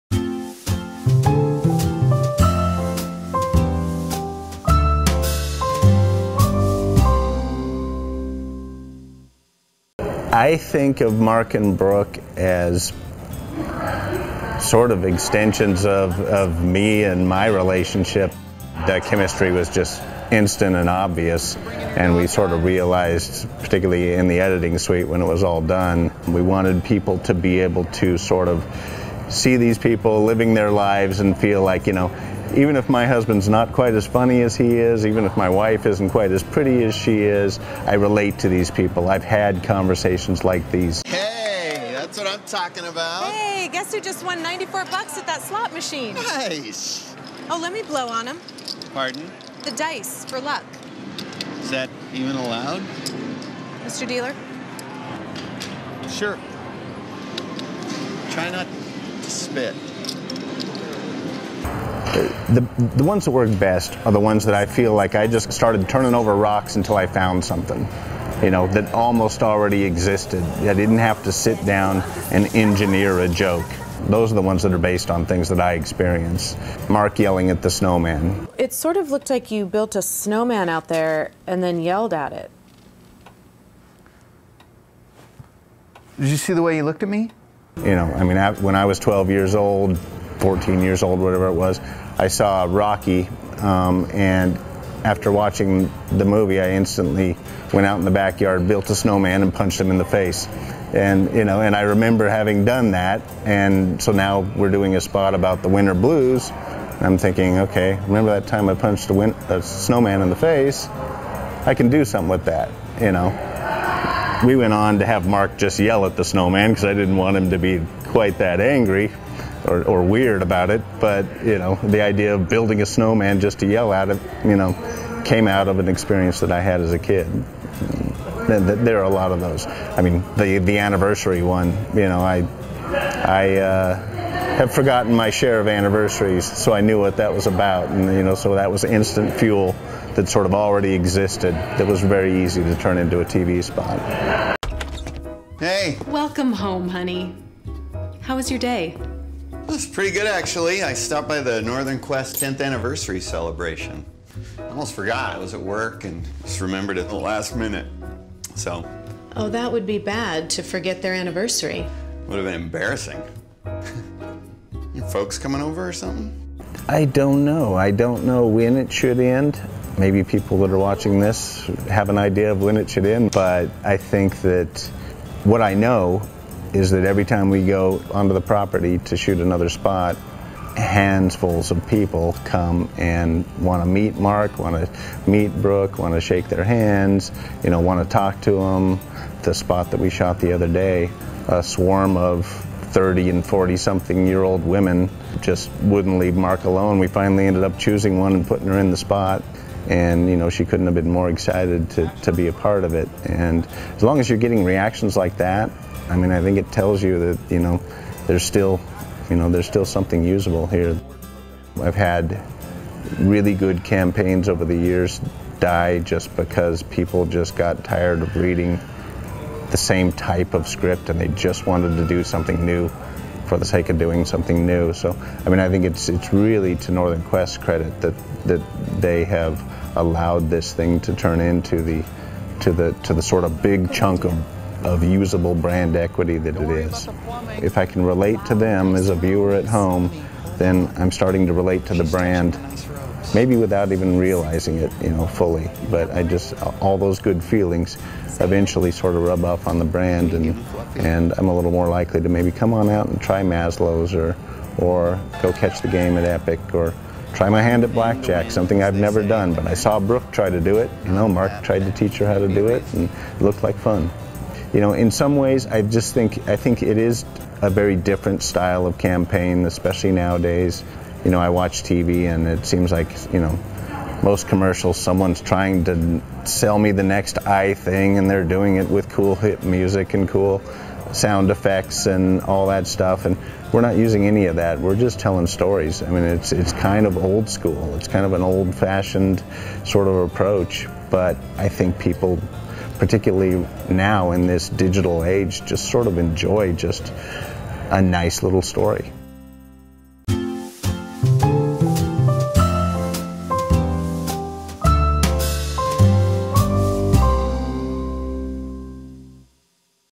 The Best of Quest: Dry Humor, Fun Times and Telling Stories
When Northern Quest's "Mark and Brooke" television campaign began two years ago, we never dreamed it would last as long as it did or inspire so many positive comments from Spokane to Missoula... (more